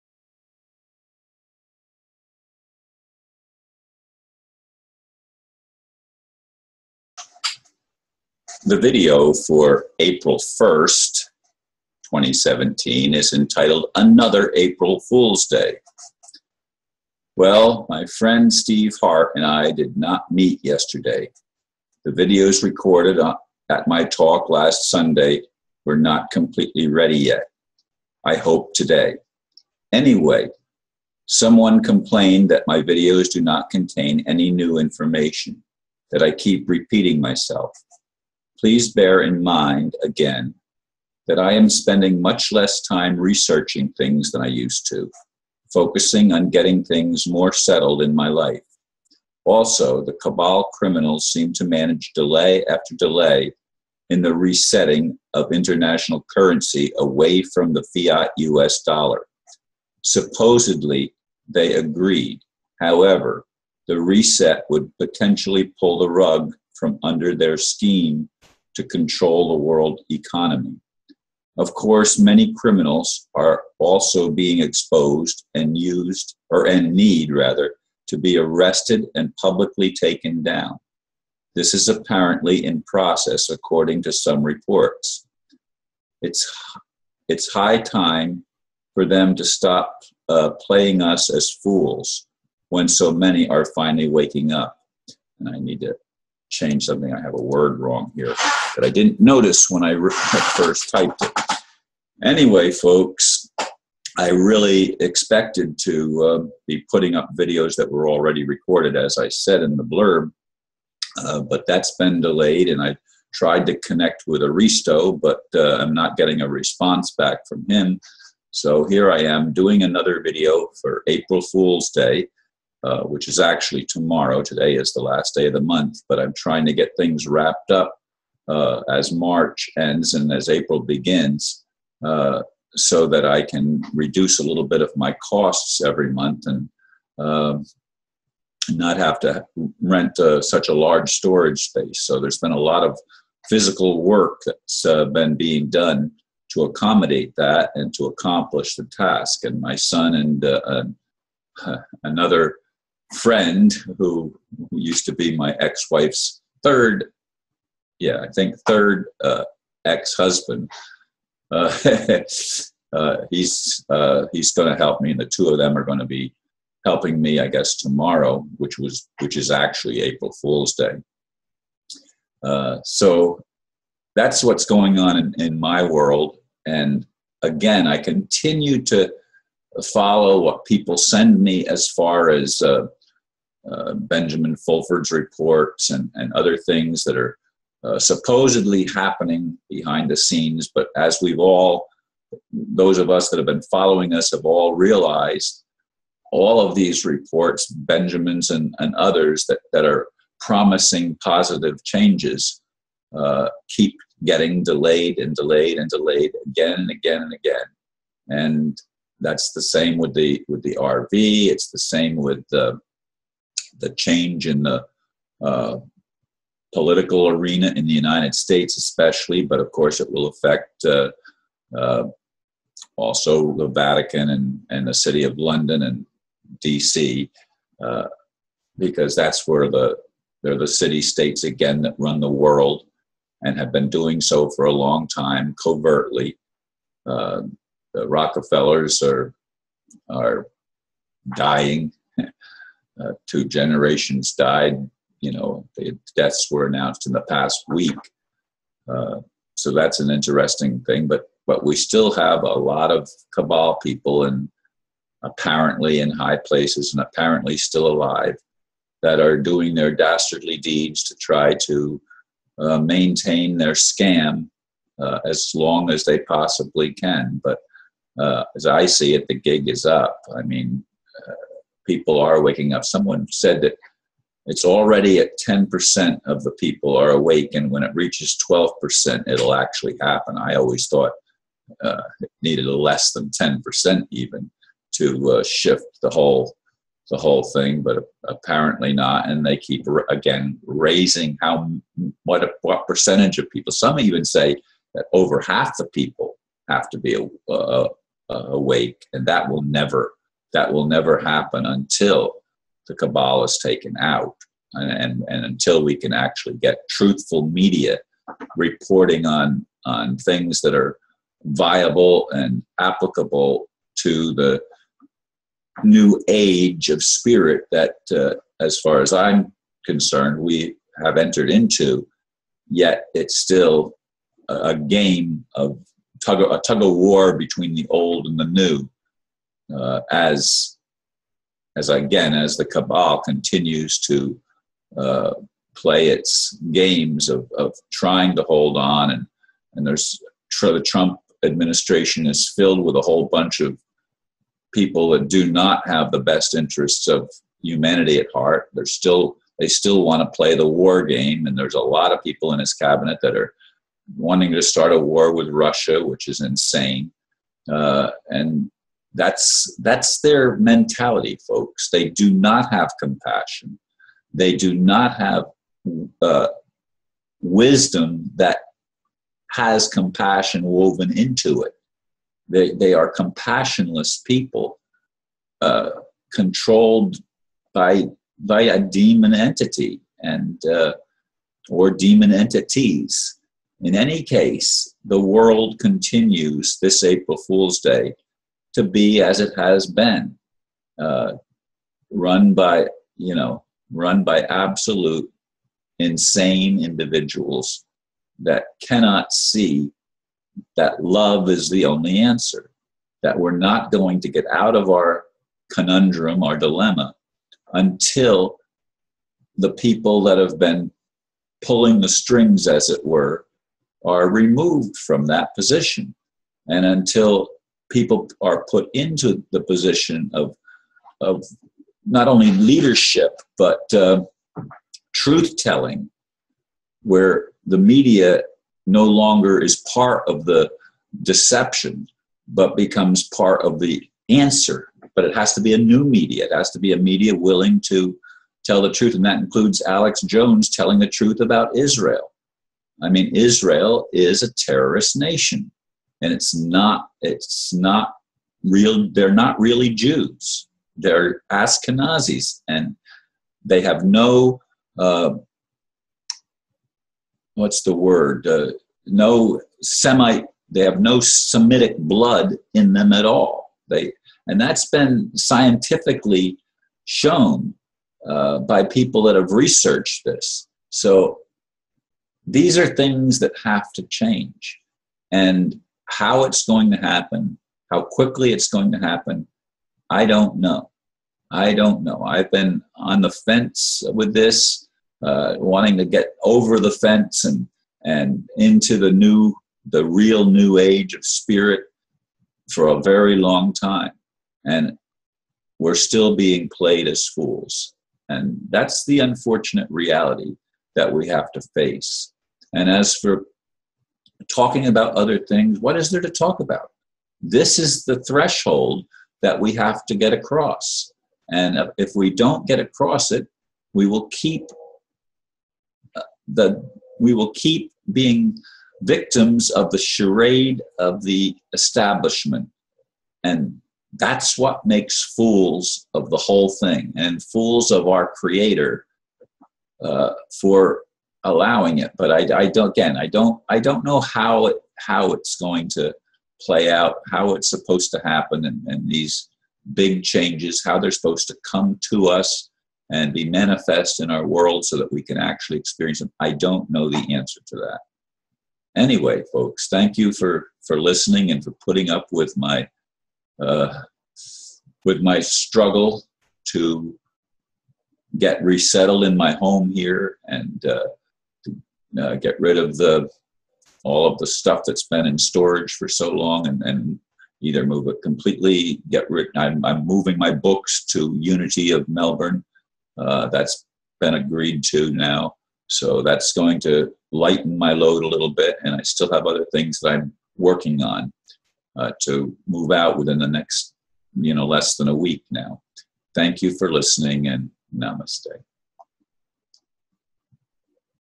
4-2-17 – IS THE CABAL BEING TAKEN OUT?
IS THE CABAL BEING TAKEN OUT?
Once again, the videos recorded at my talk last Sunday were not completely ready yet. This time they arrived in the wrong format. Also, I have been unable to reach Aristo. Therefore, I am recording yet another video – this one in response to notices I'm receiving about the so-called cabal being taken out. While I certainly hope this is the case, I won't hold my breath. This was supposed to happen a good while ago now. It obviously has not happened yet, even though there are signs that something is going on. We are told that some things cannot happen until key players are removed – the currency revaluation for example  I'm sure there is a lot happening behind the scenes; but until it is made public, there's not much anyone can say, including me. At this point, I do not know what info to trust. I am convinced that some well-meaning people are doing the best they can. Whether they will have the help and support of higher dimensional entities remains to be seen.